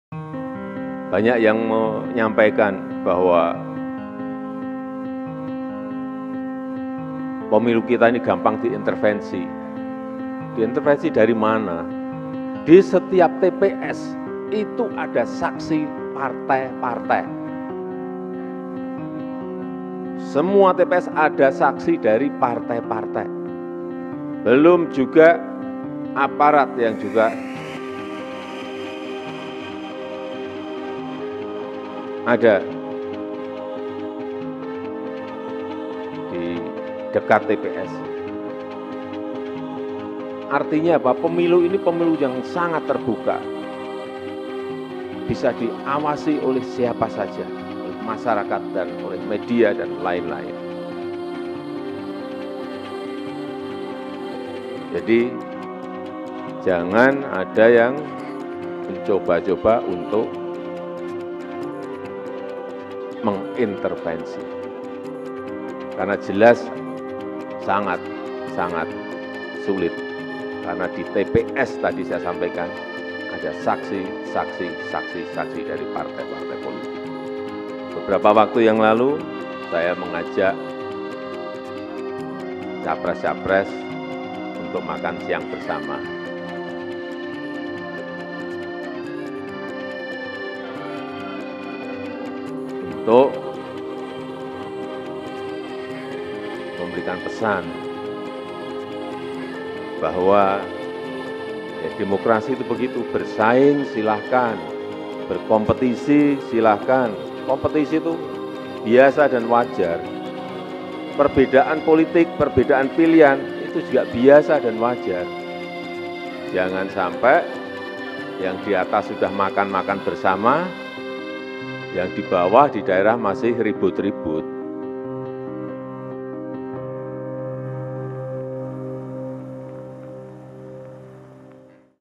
Presiden: Singgung Ajak Capres Makan Bersama, Jangan Sampai di Bawah masih Ribut
Presiden Jokowi mengatakan Pemilu 2024 menjadi yang terbesar dalam sejarah penyelenggaraan pemilu di Indonesia. Dengan 840 ribu TPS, pesta demokrasi lima tahunan itu menghadapi tantangan yang besar dalam penatakelolaannya, pengamanan dan distribusi. Dalam Rapat Koordinasi Nasional Penyelenggara Pemilu yang digelar DKPP, Presiden mengingatkan semua pihak tidak mengintervensi Pemilu 2024.
"Jadi jangan ada yang mencoba-coba untuk mengintervensi, karena jelas sangat sangat sulit," tegas Presiden, Rabu (8/11). Pernyataan Jokowi disampaikan sehari setelah MKMK menyatakan Ketua MK Anwar Usman melanggar etik berat. Anwar sendiri merupakan adik ipar dari Jokowi. "Jangan sampai yang di atas sudah makan-makan bersama, yang di bawah, di daerah, masih ribut. Ini yang perlu kita jaga bersama-sama," tandasnya.Presiden Joko Widodo DKPP Pemilu 2024 Pilpres 2024 Pemilu Damai